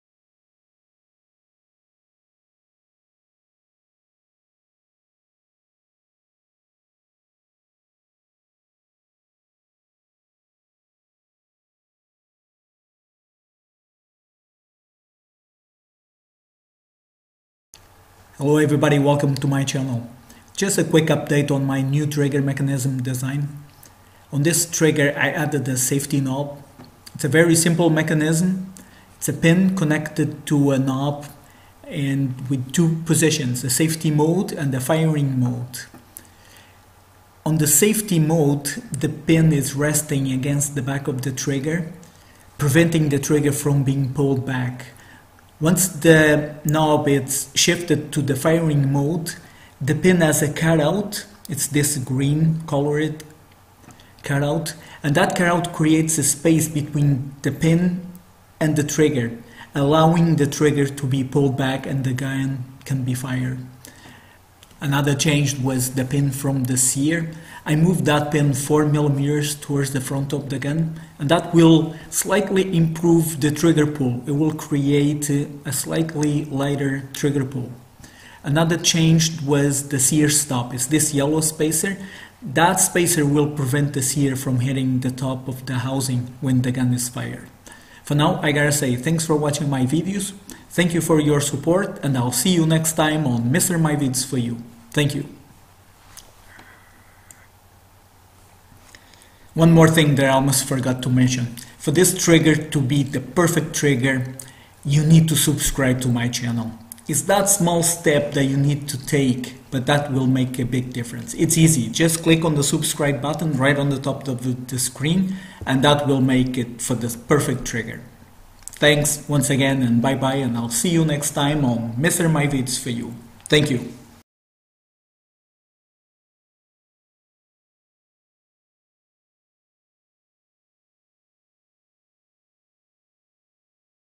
New Speargun Trigger Design with safety knob
NewSpeargun Trigger Design animation with a safety knob for a homemade speargun, this is my new trigger mechanism design with a safety knob, smoother trigger pull and a new sear stop that preventes the sear from hitting the top of the trigger housing when the gun is fired.